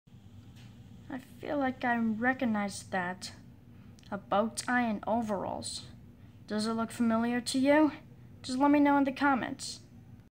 Does this outfit look familiar to you? (part 2)
It's a character with overalls, and a bow tie with polkadots, who wears this?